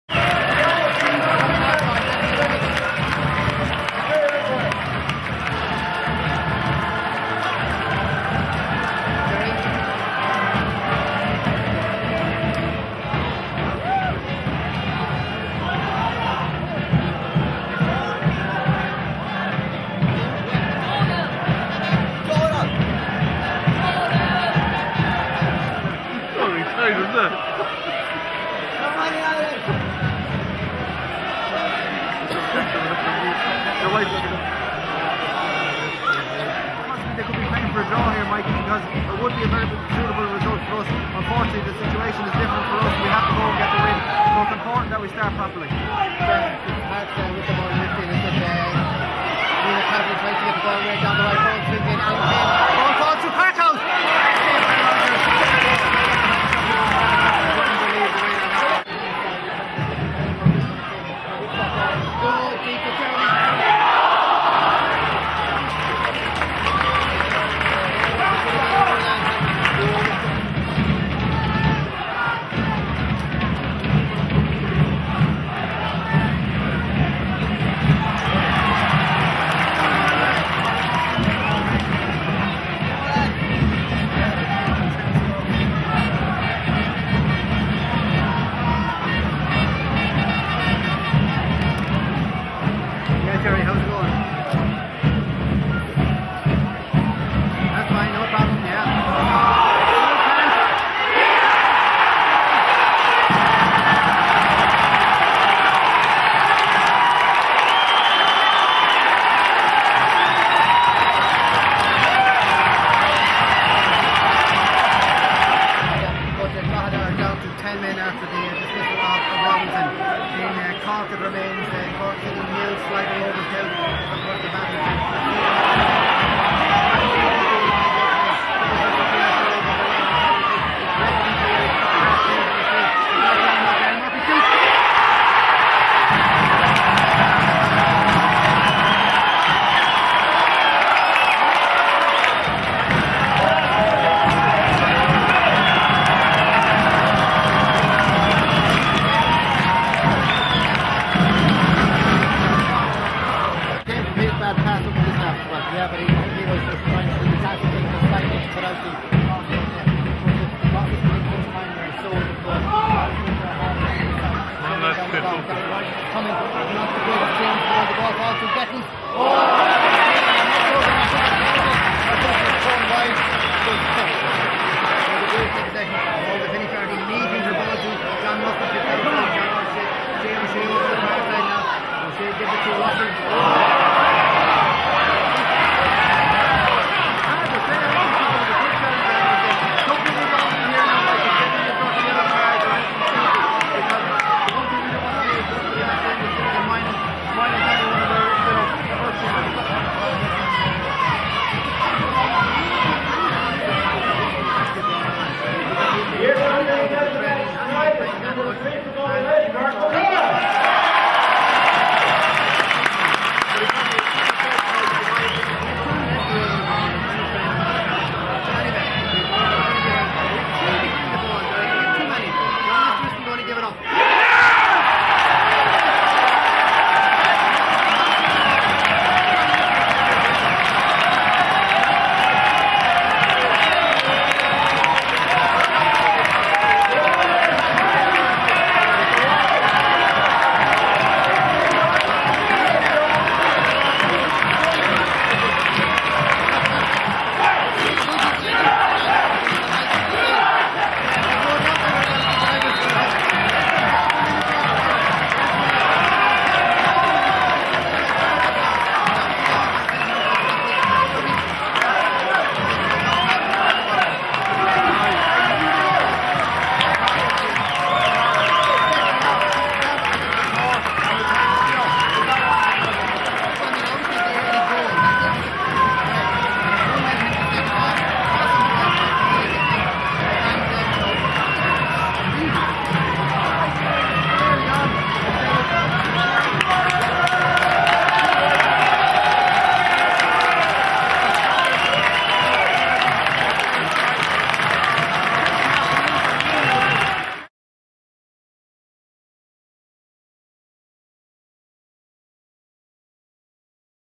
Galway United 3-0 Finn Harps
Played in Terryland Park, Galway on the 7th November 2008.